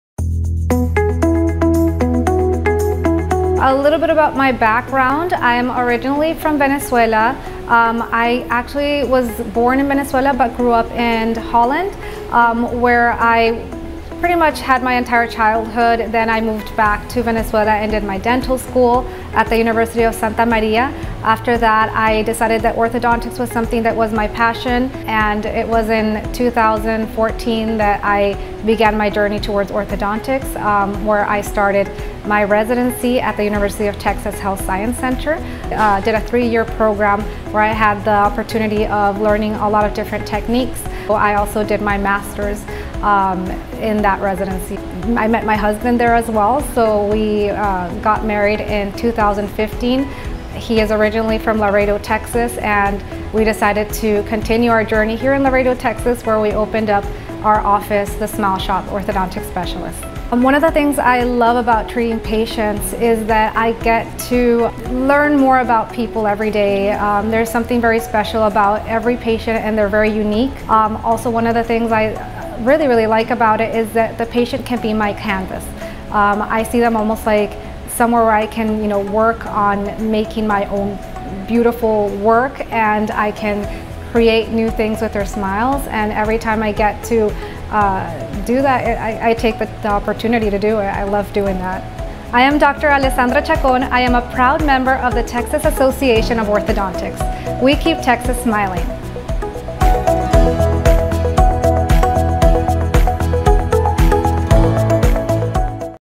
Meet Dr. Alessandra Chacon, TAO member orthodontist
Dr. Alessandra Chacon, originally from Venezuela, loves watching her patients' transformation as they go through their treatment. She feels inspired by the artistry involved in orthodontics and enjoys how every patient is unique and special.

A la Dra. Alessandra Chacón, originaria de Venezuela, le encanta ver la transformación de sus pacientes a medida que se someten a su tratamiento. Ella se siente inspirada por el arte involucrado en la ortodoncia y disfruta de cómo cada paciente es único y especial.